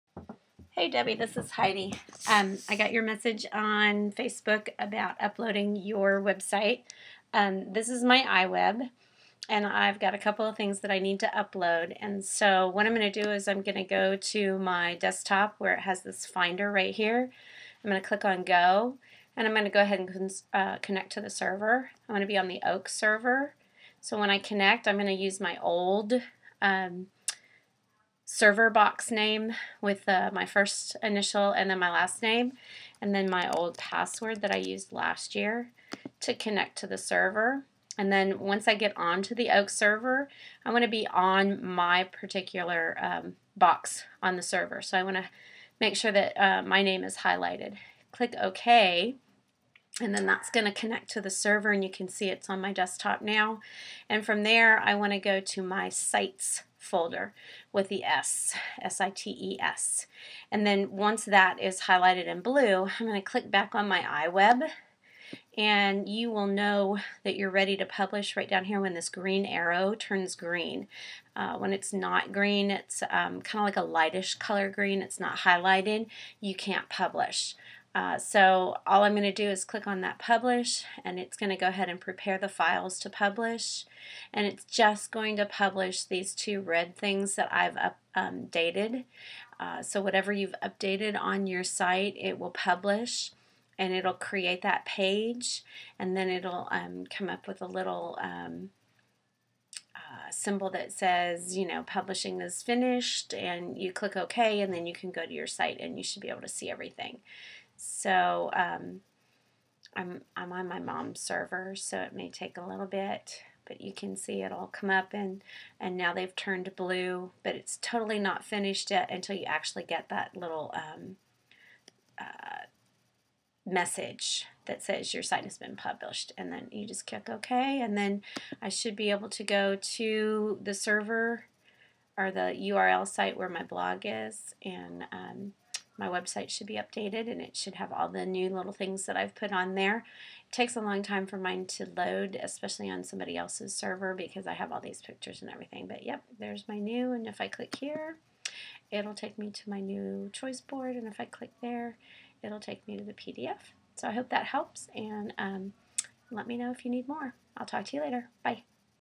upload to iWeb
Debbie...here's a quick tutorial on how to upload your iWeb to the server at school